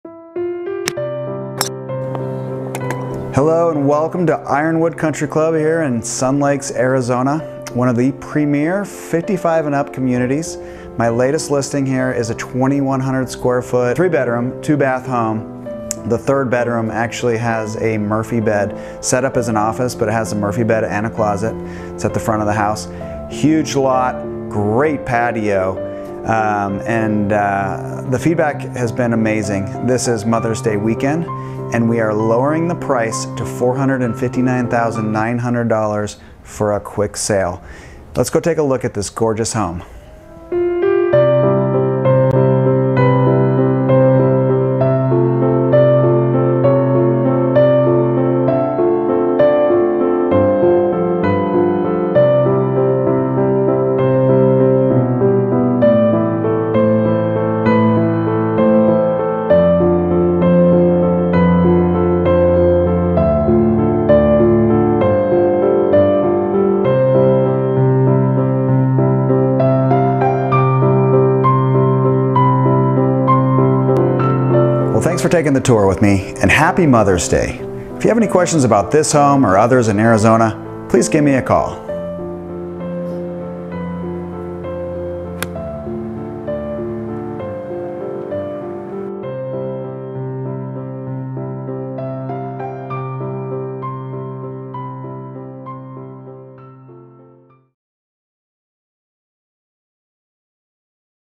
3 bed 2 bath in Ironwood CC of Sun Lakes in Chandler Arizona
This gorgeous home is NOW available - but with a new price of $459,900 we don't expect it to last long.

Are you interested in buying or selling in Arizona? Give me a call.

Ryan Barmore
REALTOR
Lucas Real Estate